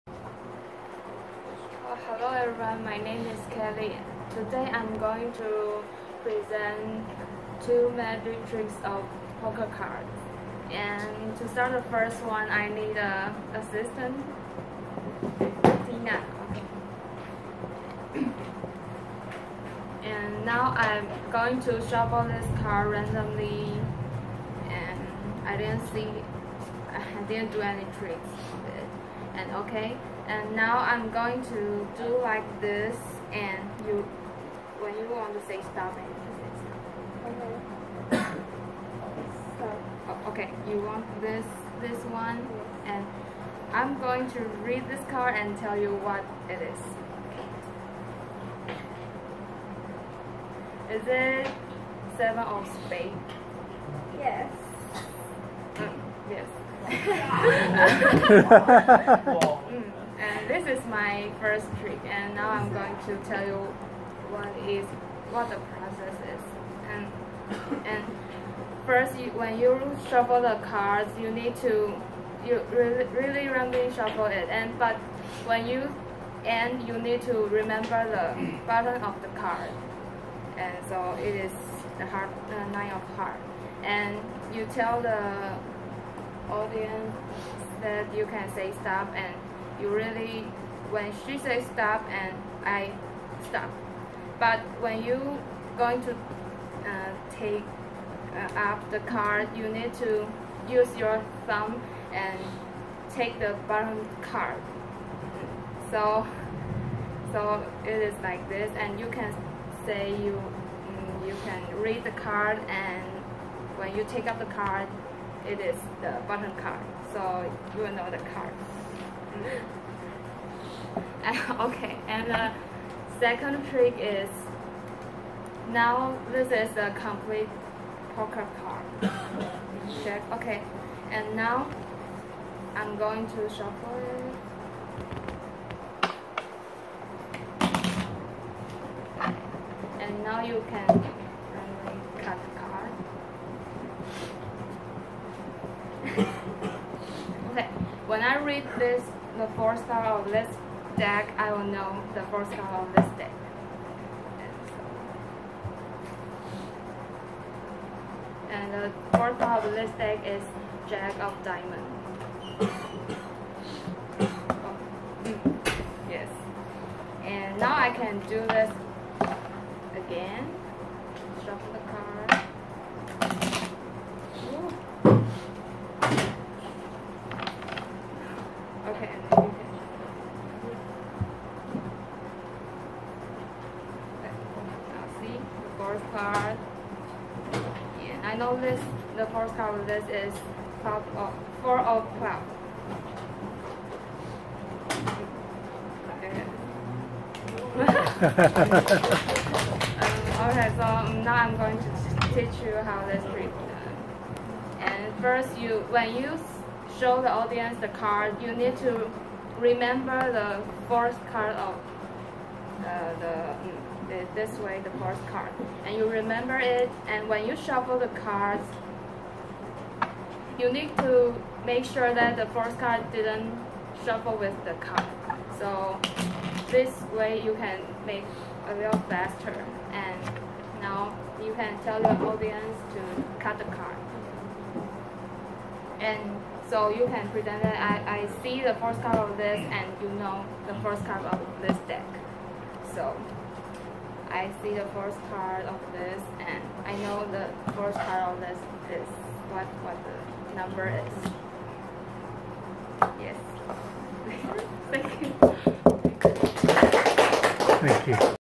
Kelly Lin Magic Lesson
Kelly Lin teaches us a lesson in magic trick performance. SPARKSHOW: Magic Voices. In this issue of SPARKSHOW, students present a lesson on how to perform 1-3 magic tricks. Please note, any views expressed in this video are those of the person expressing them and do not represent those of the instructor, the English Department, or National Chengchi University. for more about the English Department at NCCU.